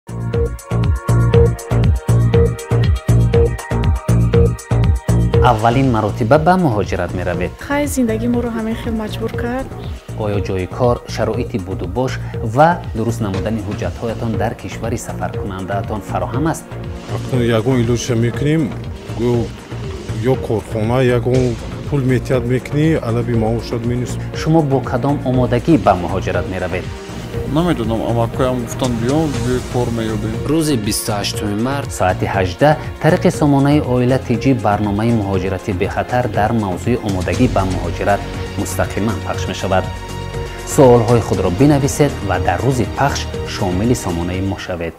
Муҳоҷирати бехатар: Савол дорӣ, занг зан!
Шумо ба муҳоҷирати меҳнатӣ рафтаниед, вале аз қонунҳои муҳоҷират хабар надоред?
Намедонед, ки муҳоҷир бояд кадом ҳуҷҷатҳоро омода намояд?
Мехоҳед донед, ки дар сурати музди меҳнати муҳоҷиронро сари вақт надодани корфармо чи кор кардан лозим аст?
Аз савдои одамону дигар хатарҳои муҳоҷират ва роҳҳои пешгирии чунин ҳодисаҳо хабардор шудан мехоҳед?
Пас биштобед!
Рӯзи 28-март, соати 18.00 дар идораи сомонаи Оила. ТЧ дар мавзӯи “Муҳоҷирати бехатар: омодагӣ ба сафар” бо иштироки коршиносони соҳаи муҳоҷират Пехши мустақим баргузор мегардад. Саволҳои худро шумо метавонед пешакӣ ё дар рафти барнома ирсол намуда, аз коршиносон ҷавобҳои дақиқ ва маслиҳатҳои судманд бигиред!

Маълумоти пурраро дар бораи муҳоҷирати бехатар, дар ссылкаи зерин пайдо карда метавонед